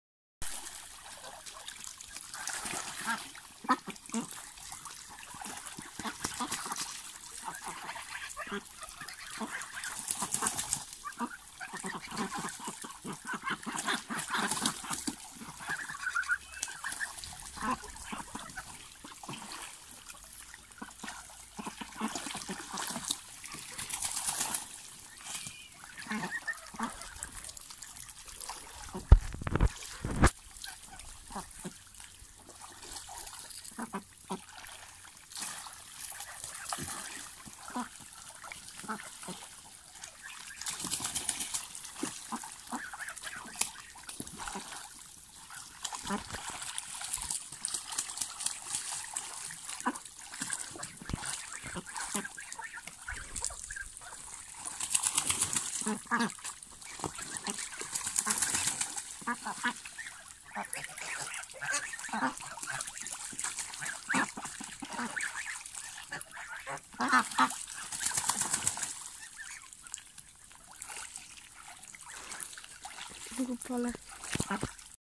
Wspomnienie z wakacji .... - gąski i kaczuszki pływające w stawie / Vlog #78
Hej, witam was w kolejnym odcinku na moim kanale tym razem jest to film, w którym pokazuje wam jak jeszcze w wakacje nasze kaczuszki i gąski kąpały się w stawie. Jest to też przedostatni film z tej mini-serii "wspomnienia z wakacji...". Od następnego wtorku będą się już pojawiać filmy aktualne.
Już w piątek ostatni film z tej mini-serii :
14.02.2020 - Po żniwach